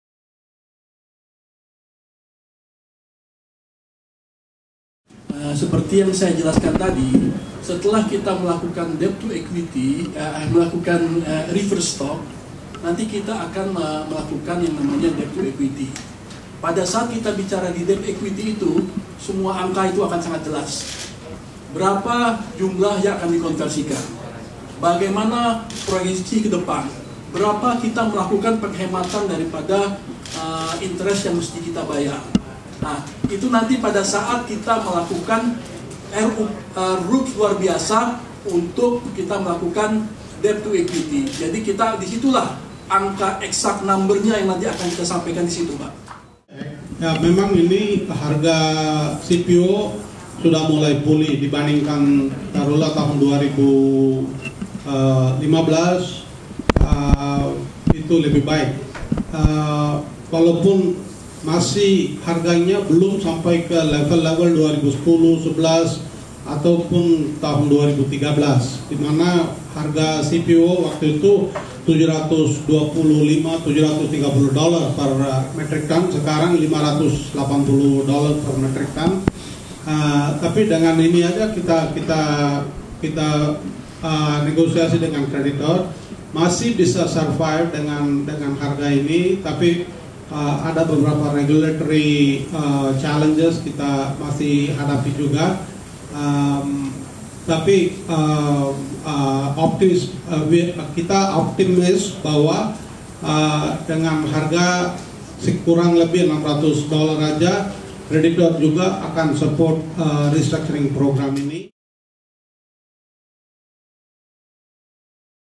Bakrie Plantation: reducing interest expenses
JAKARTA - PT Bakrie Sumatera Plantation Tbk (UNSP) plans to reduce their interest expenses around Rp350 billion through reverse stock with the ratio of 1:10. The reverse stock will be decided in extraordinary general meeting of shareholders on October 31, 2016.

Director of Bakrie Sumatera Plantation, Andi Setianto, said the management chose reverse stock in order to increase their stock price in market. "Since 2013, the stock price has been stable at Rp50 per share. A lot of investors has offered in the market but no one bids. So, we conducted a consultation with the creditors for restructuring the stock price and they agreed," he said in the company's incidental public expose,Jakarta.

According to him, they will pay the interest expenses in first quarter 2017. In the first semester 2016, the company's interest expenses noted Rp351 billion and in the 2015 noted Rp611.7 billion. Bakrie

Sumatera Plantation's stock price has been decreasing since 2010 as noted at Rp390 per share, which continue falling to Rp285 per share in 2011, and drop to Rp83 per share in 2012. (LK)